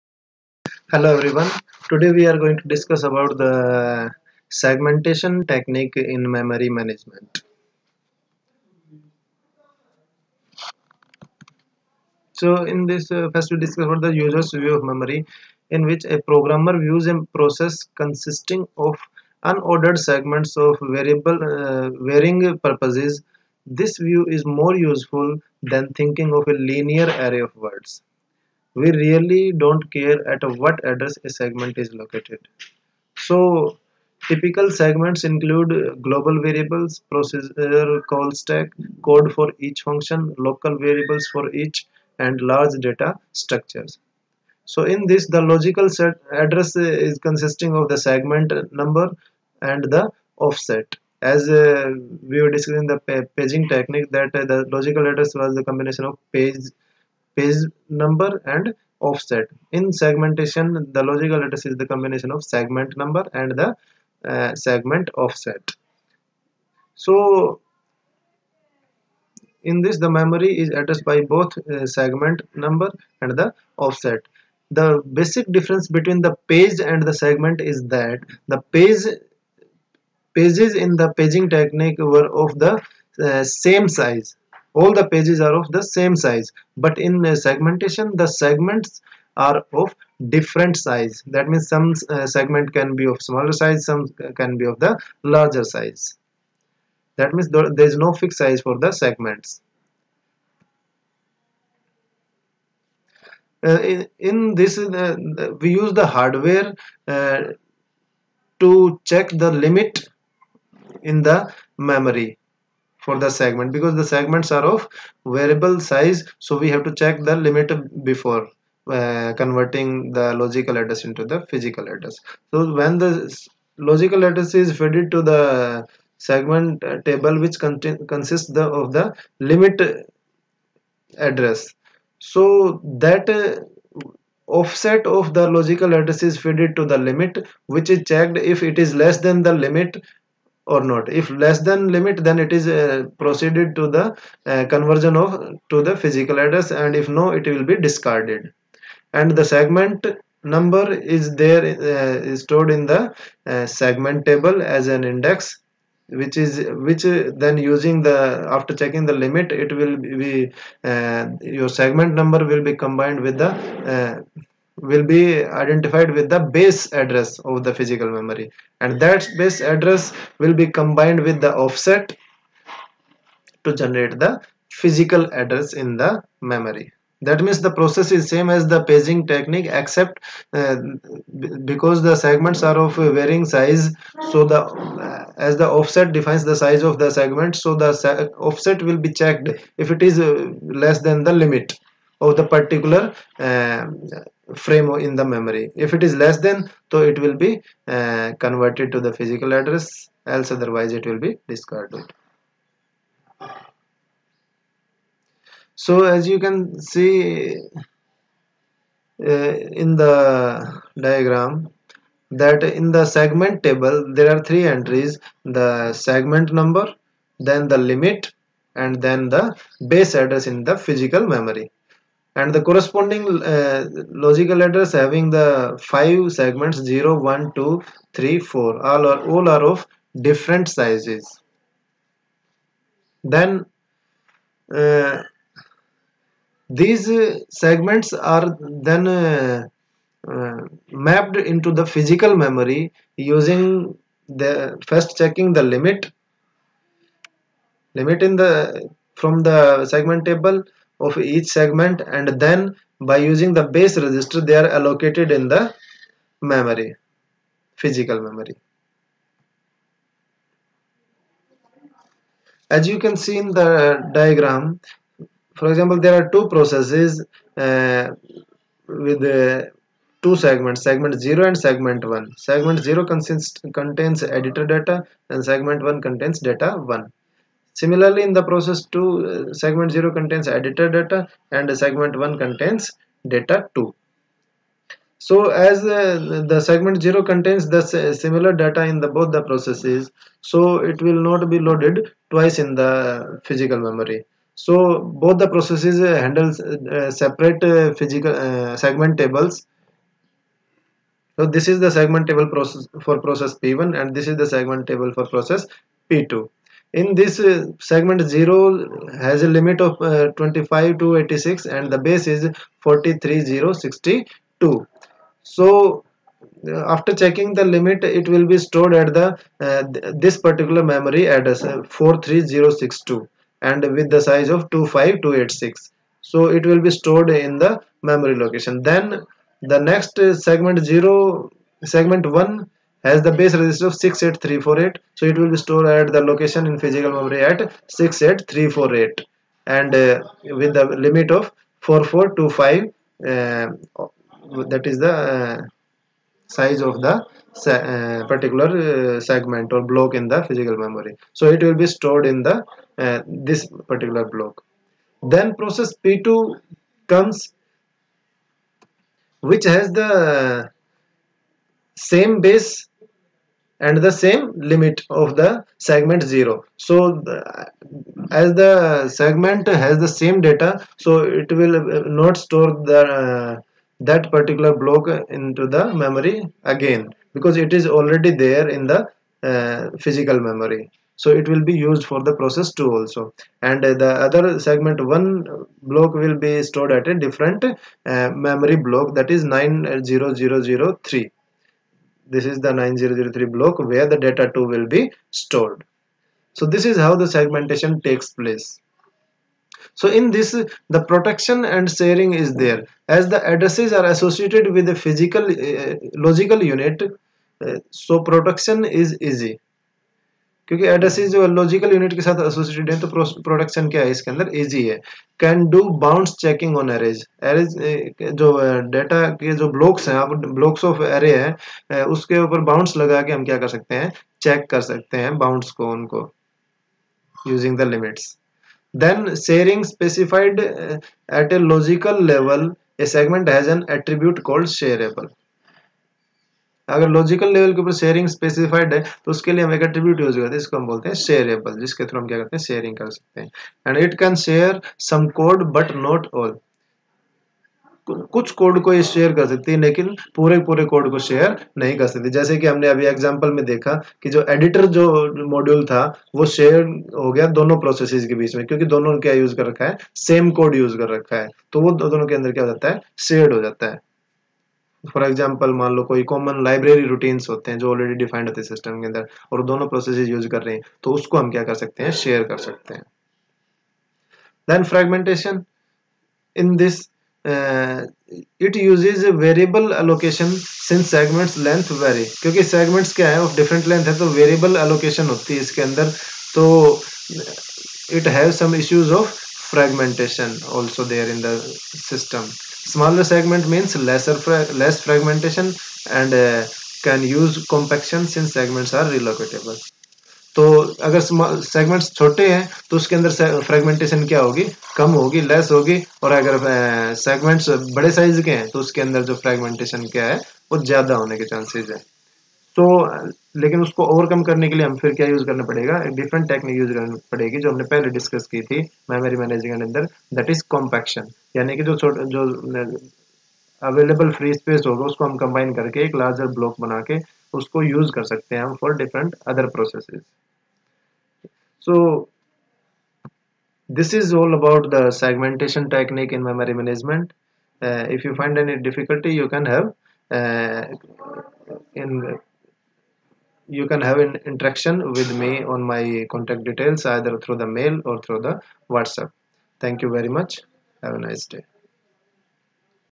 segmentation technique in memory management by Dr vatan sehrawat
this video discuss about segmentation technique in memory management by Dr vatan sehrawat